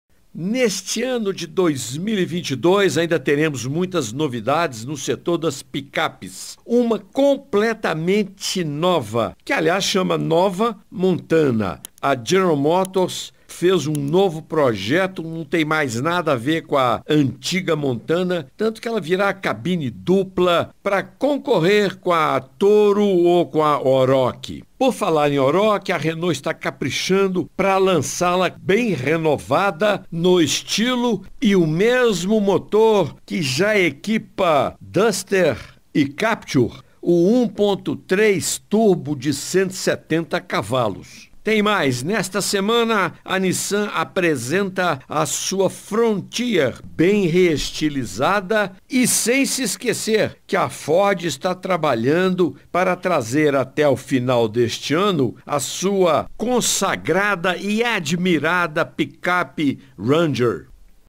Saindo do forno: picapes da GM, Renault, Nissan e Ford
Segmento de caminhonetess no Brasil será contemplado com quatro novos modelos que estão previstos para chegar ao mercado ainda em 2022.